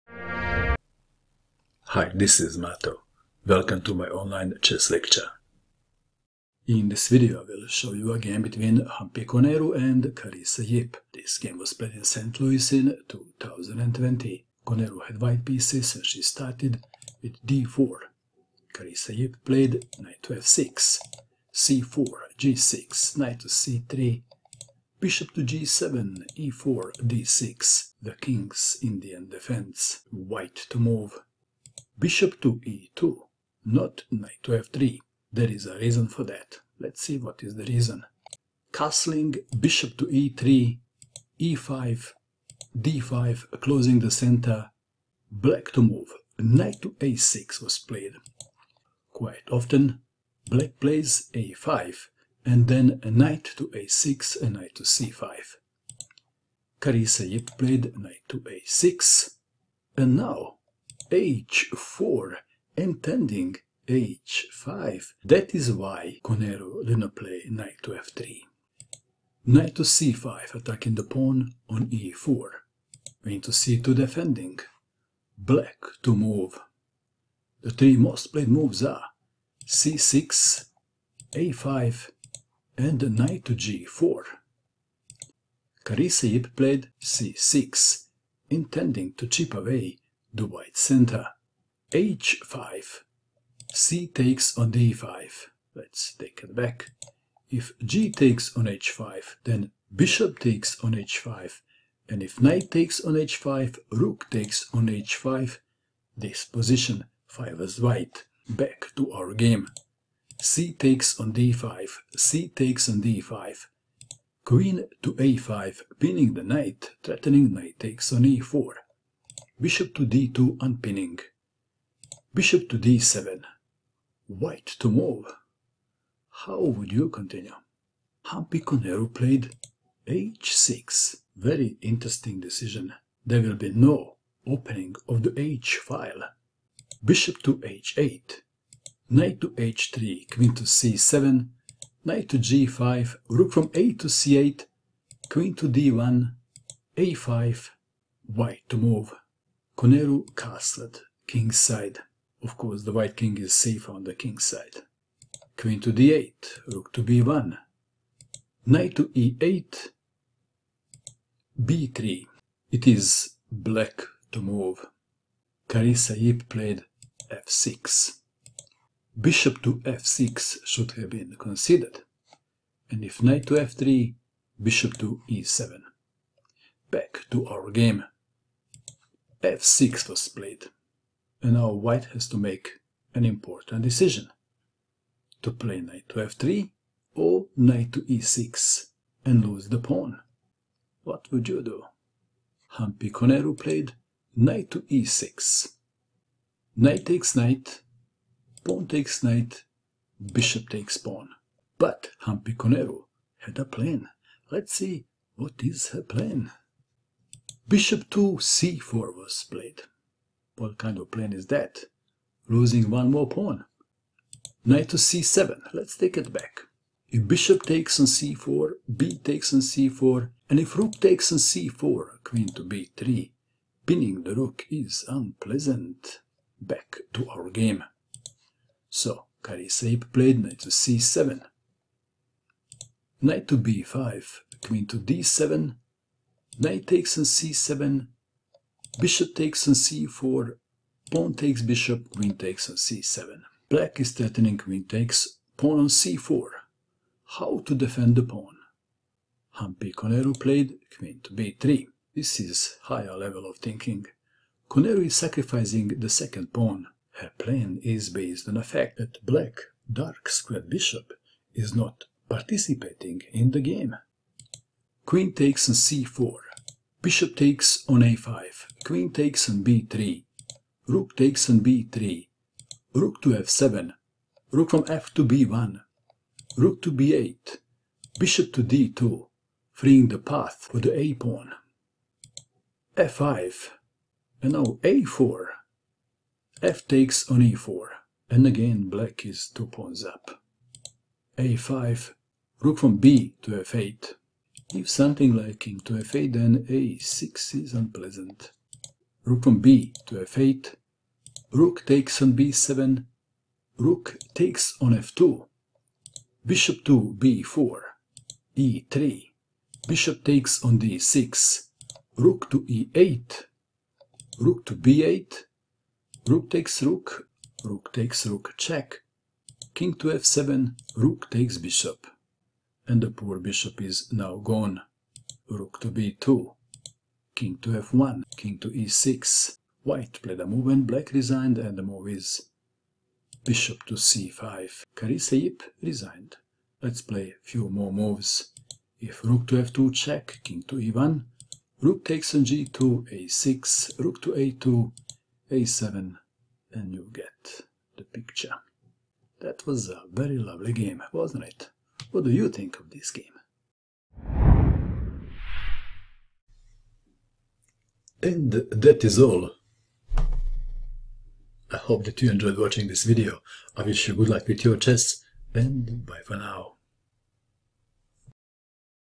Cairns Cup 2020 | Humpy Koneru vs Carissa Shiwen Yip: Cairns Cup 2020, St Louis
Humpy Koneru vs Carissa Shiwen Yip
Cairns Cup 2020, St Louis, MO USA, round 1
King's Indian Defense: Semi-Averbakh System (E73)  ·  1-0
1. d4 Nf6 2. c4 g6 3. Nc3 Bg7 4. e4 d6 5. Be2 O-O 6. Be3 e5 7. d5 Na6 8. h4 Nc5
9. Qc2 c6 10. h5 cxd5 11. cxd5 Qa5 12. Bd2 Bd7 13. h6 Bh8 14. Nh3 Qc7 15. Ng5
Rac8 16. Qd1 a5 17. O-O Qd8 18. Rb1 Ne8 19. b3 f6 20. Ne6 Nxe6 21. dxe6 Bxe6
22. Bc4 Nc7 23. Nb5 Qd7 24. Nxc7 Bxc4 25. bxc4 Qxc7 26. Qb3 Qxc4 27. Bxa5 Qxb3
28. Rxb3 Rf7 29. Rfb1 Rb8 30. Bd2 f5 31. a4 fxe4 32. a5 Rbf8 33. Rxb7 Rxf2 34.
Bb4 e3 35. Bxd6 Re8 36. Rb8 Rxb8 37. Rxb8+ Kf7 38. Rxh8 Rb2 39. Kf1 Ke6 40. Bc5
1-0

Free Swiss Online Tournament
Round Robin
Swiss System
Single Elimination